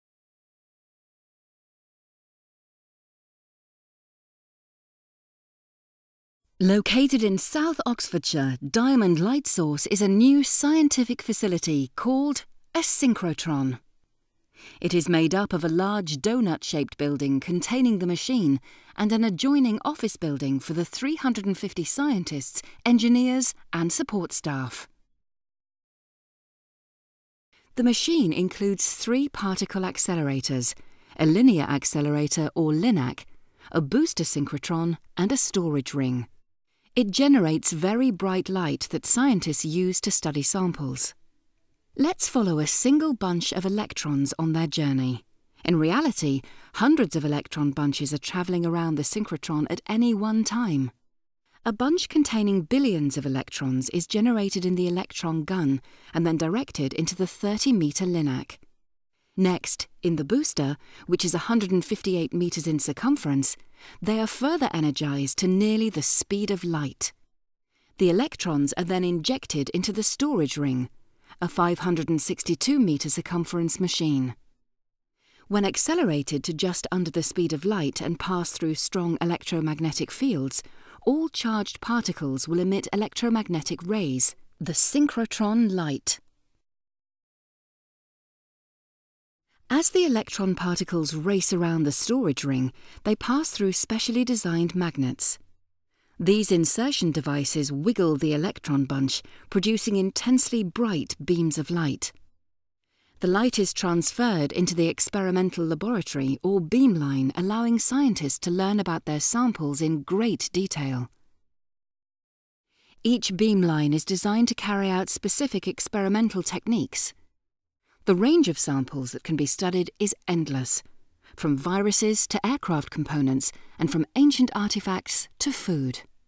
What is the Diamond synchrotron?
An overview of the synchrotron - how electrons are accelerated around the synchrotron, and how they interact with special magnets to generate synchrotron light